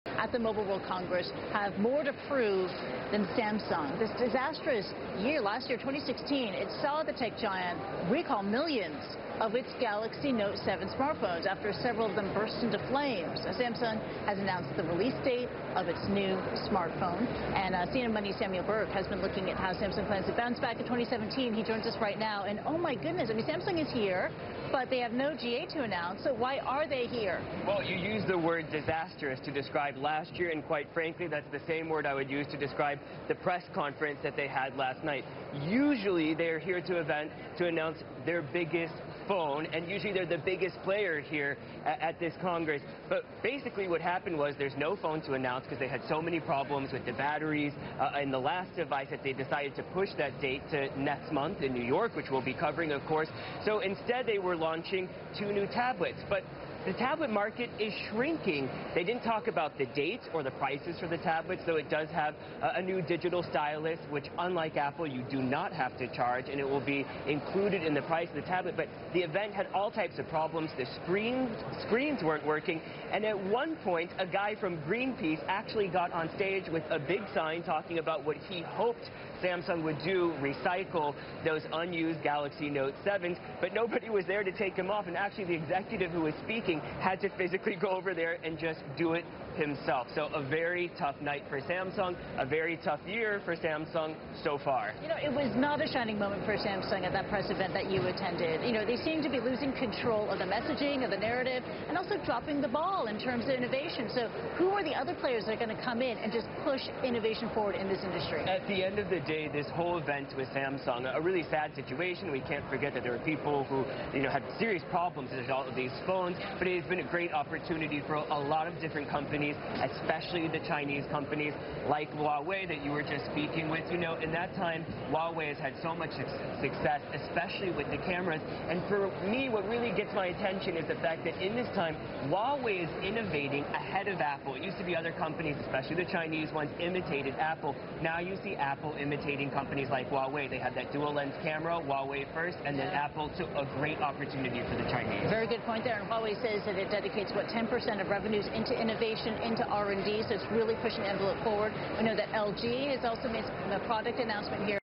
Samsung stumbles at Mobile World Congress
Samuel Burke tells Kristie Lu Stout about Samsung's error-strewn press conference in Barcelona